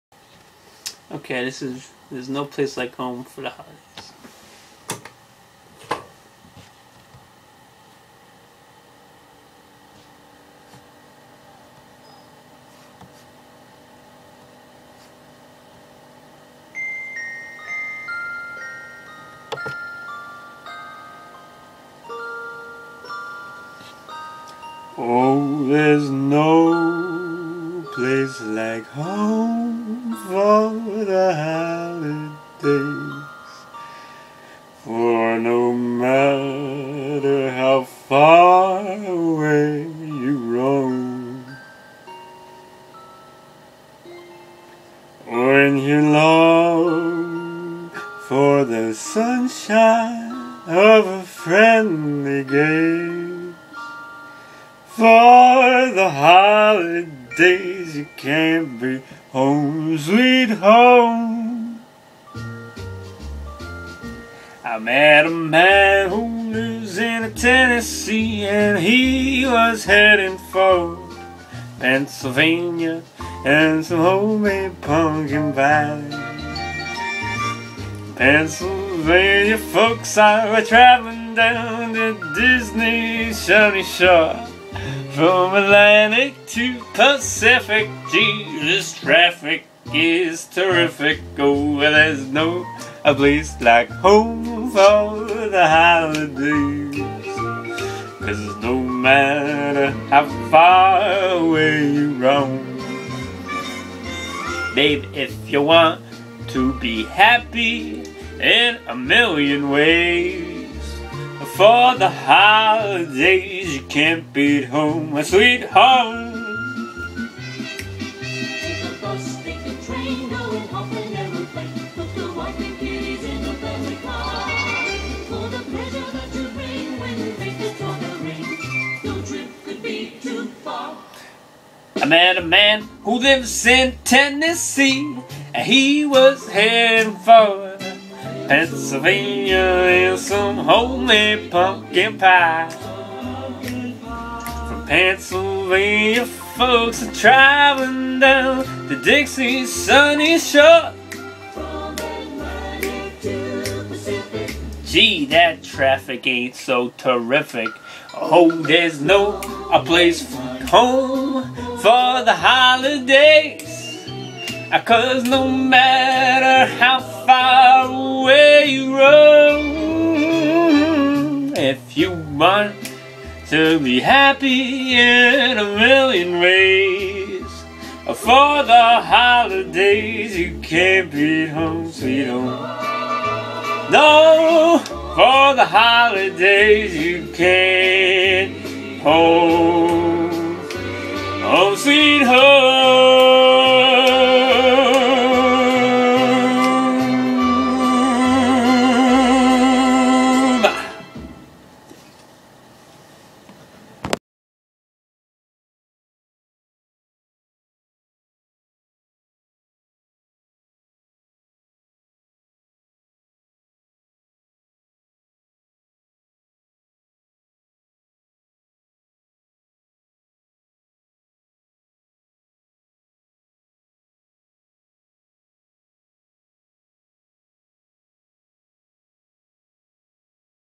Home for the Holidays
I'm posting this now in celebration of our country bringing the Iraq troops home for the holidays.